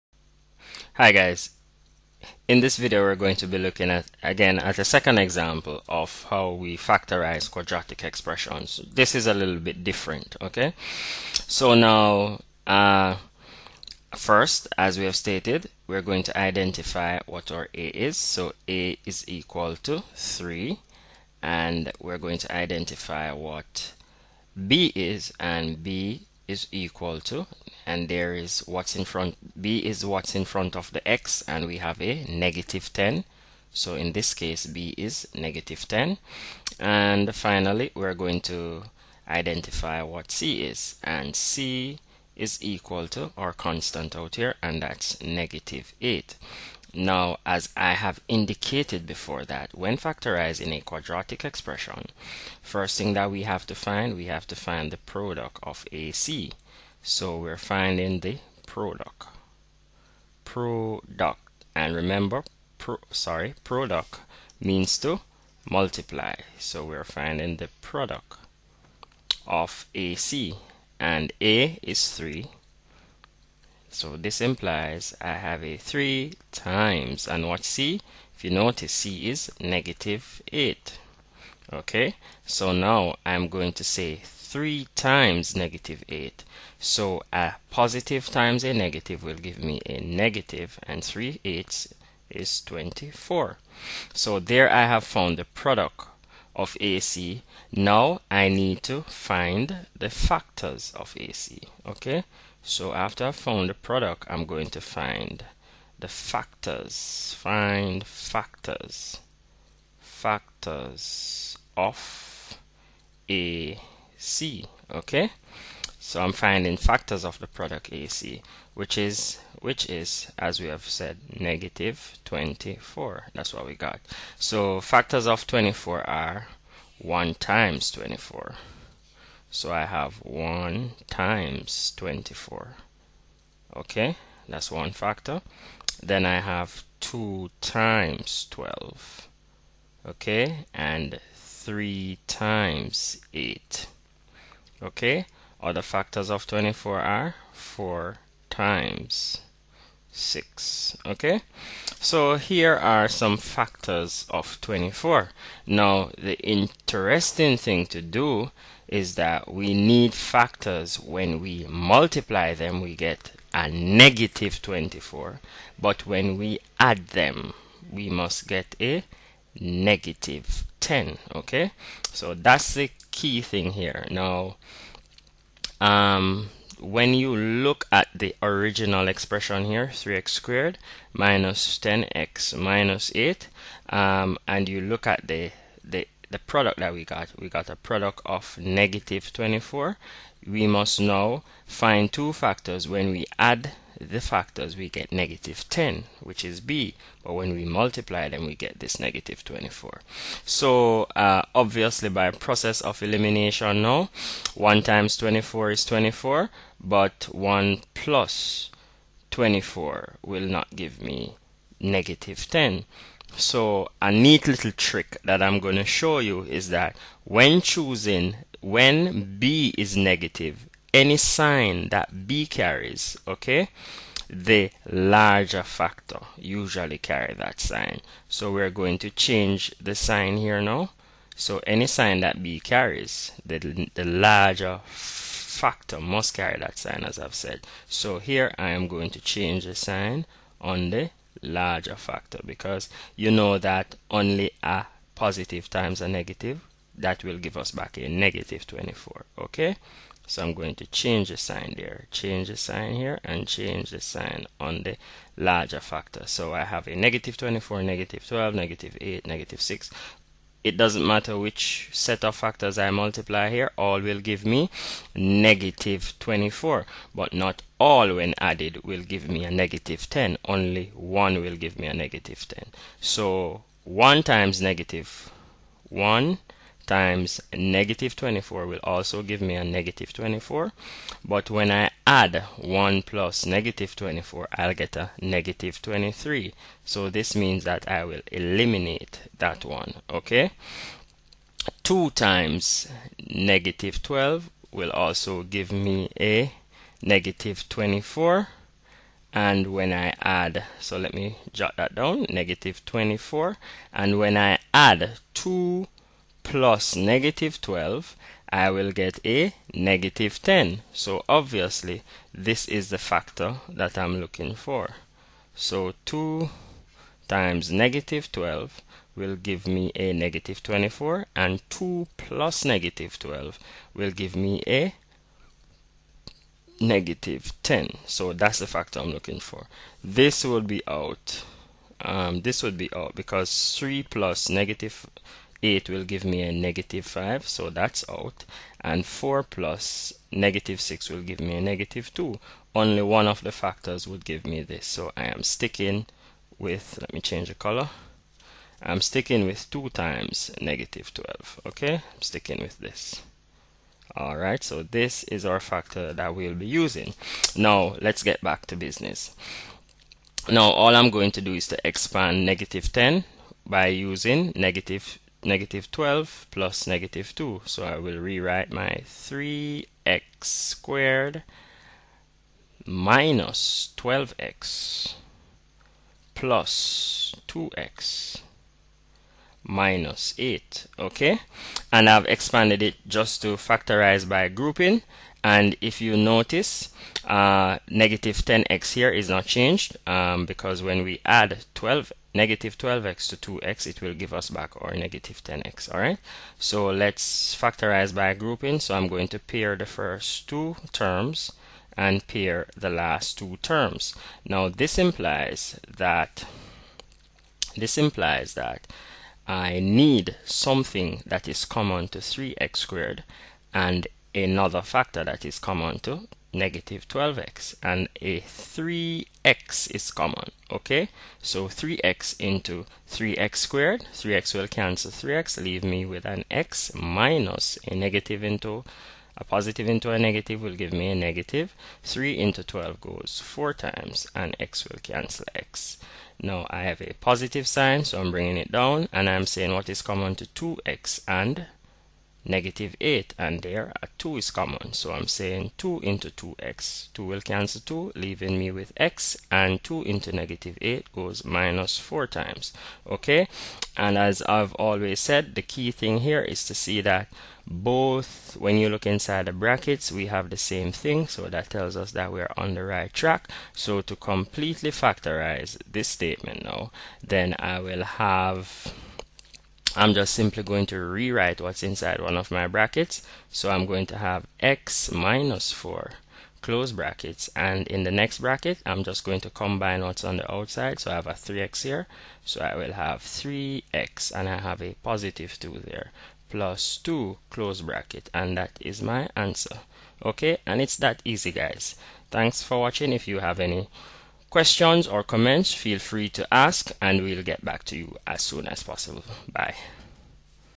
Quadratic Factorization_Part 2_by Will EduTech
Showing how to factorize different types of quadratic expressions.